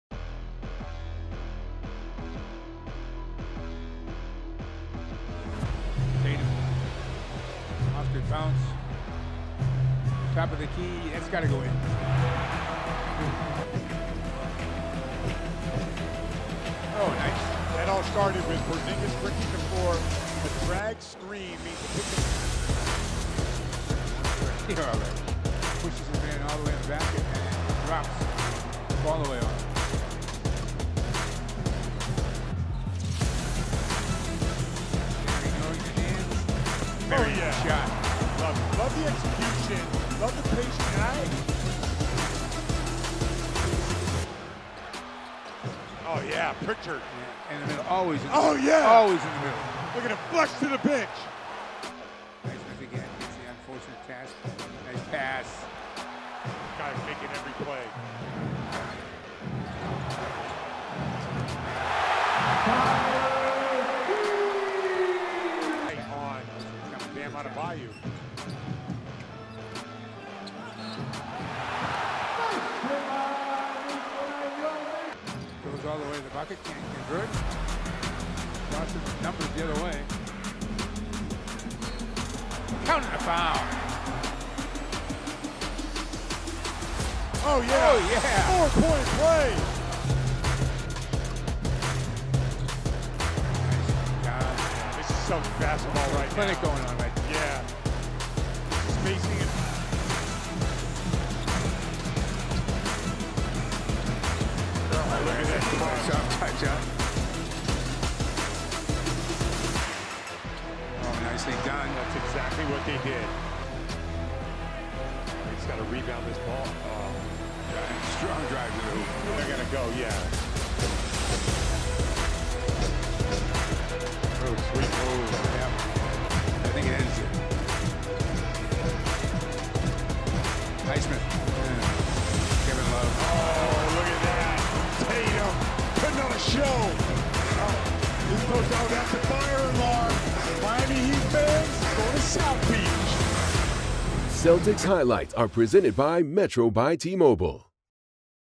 FULL GAME HIGHLIGHTS: Celtics beat Heat handily 104-84; Take 2-1 series lead
Celtics were in control all game. Jayson Tatum continually made the right play and led the Celtics to victory. The C's now have a 2-1 series lead.